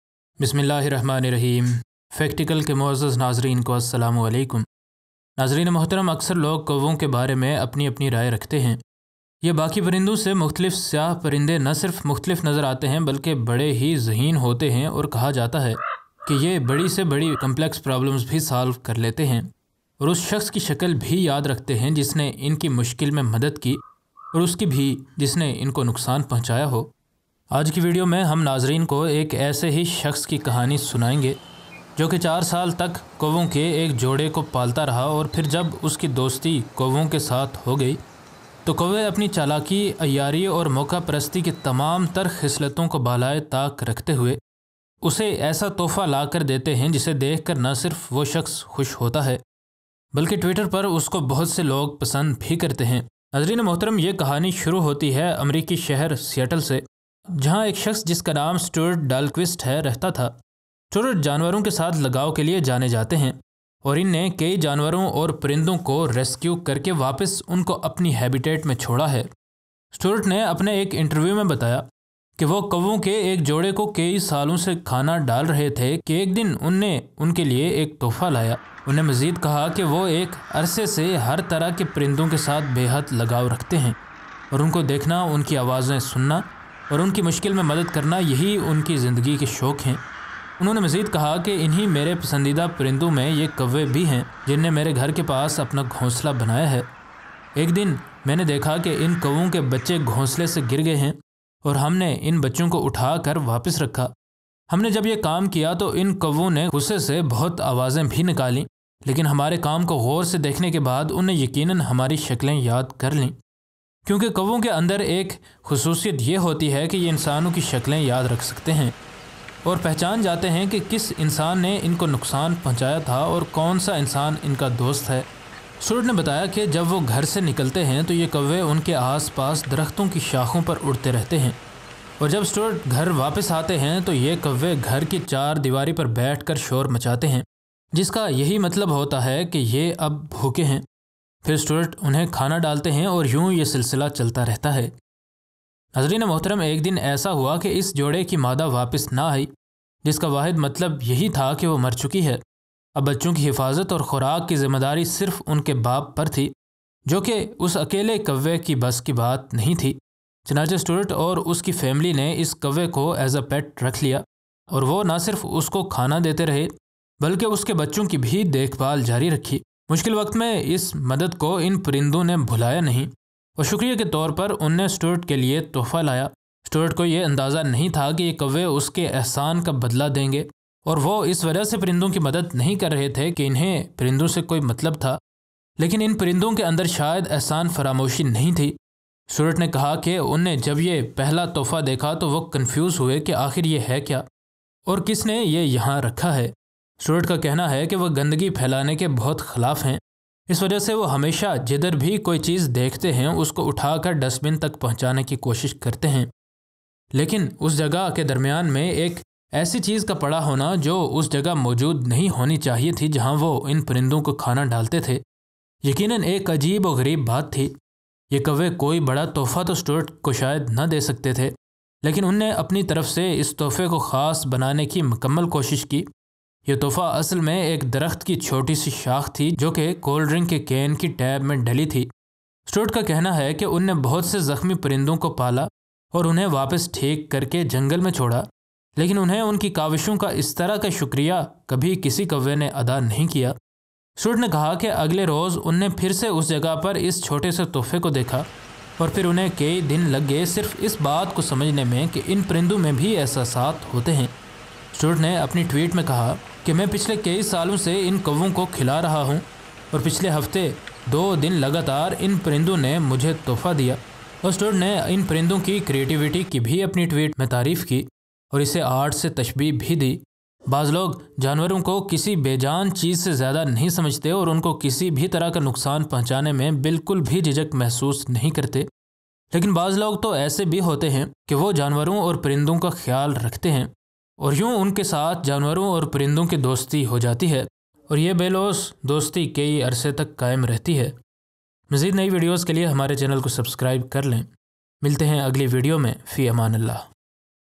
Crows Became Friend With a Person and In Return, They Brought For Him This Gift
Crows became friends with a person and in return, they brought for him this gift.

For Brand Promotion and other Queries